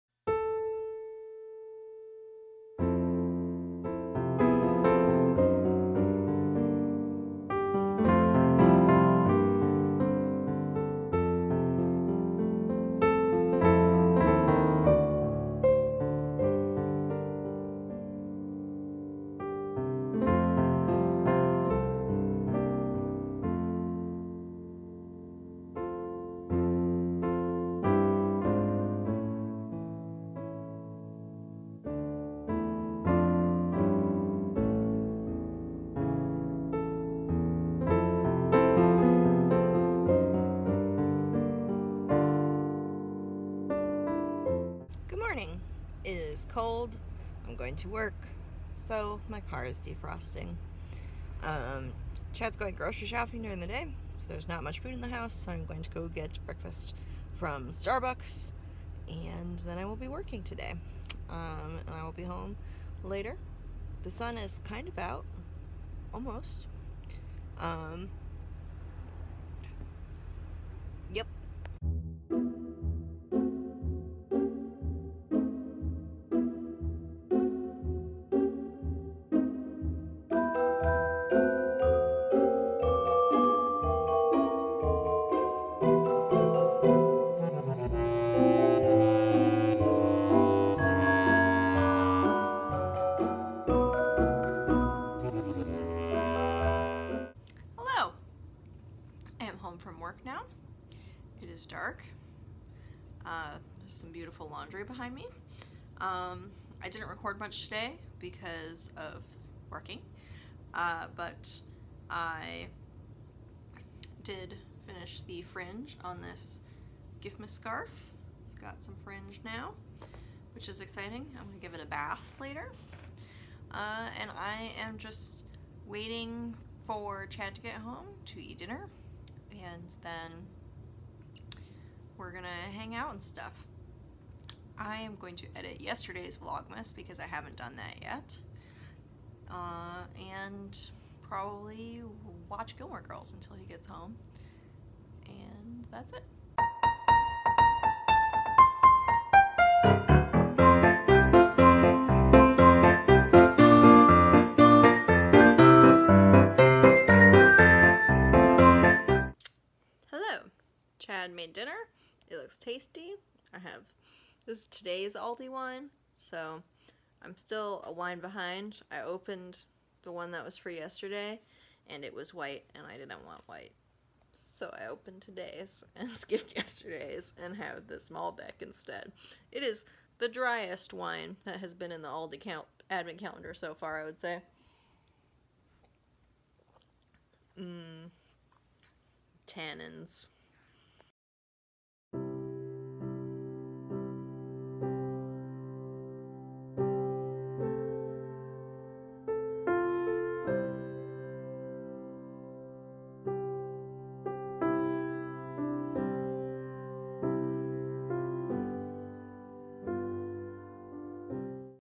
Vlogmas 2019 - 11: Work, Embroidery, and Aldi Wine.
It's a go into the office day. I open Advent Calendars, twist the fringe on a scarf, eat dinner, and work on an embroidery project for a friend.